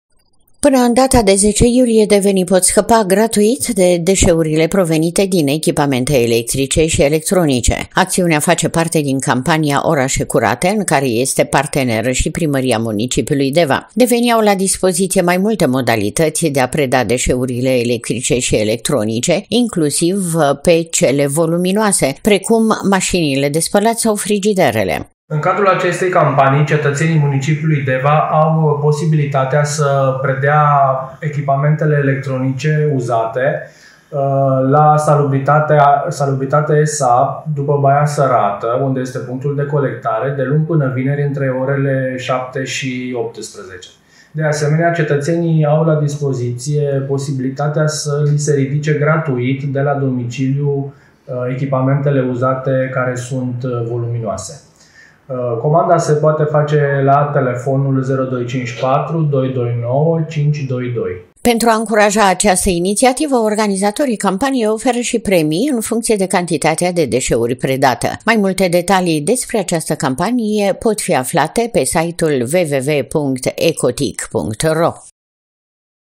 DEVA: CAMPANIE DE COLECTARE GRATUITĂ A DEȘEURILOR ELECTRICE ȘI ELECTRONICE
Devenii au la dispoziție mai bine de o lună pentru a scăpa, gratuit, de deșeurile provenite din echipamente electrice și electronice. Acțiunea face parte din campania “Orașe curate”, la care au aderat mai multe municipalități și societăți de salubritate din țară.